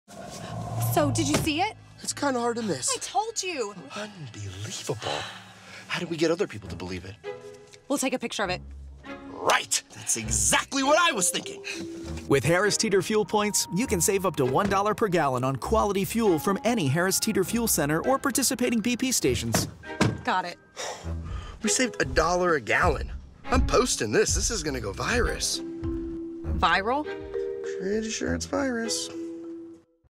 HT Fuel Points - Did You See It?
It’s easy to save on fuel with Harris Teeter’s new Fuel Points program. Just use your VIC card every time you shop and you’ll earn one fuel point for every dollar you spend. For every 100 points you earn, you can save ten cents per gallon on fuel purchases at any Harris Teeter fuel center and participating BP or Amoco locations. The more points you earn, the more you save…up to one dollar per gallon!

(Up to 35 gallons. Exclusions Apply - visit harristeeter.com for full details)

HT app Links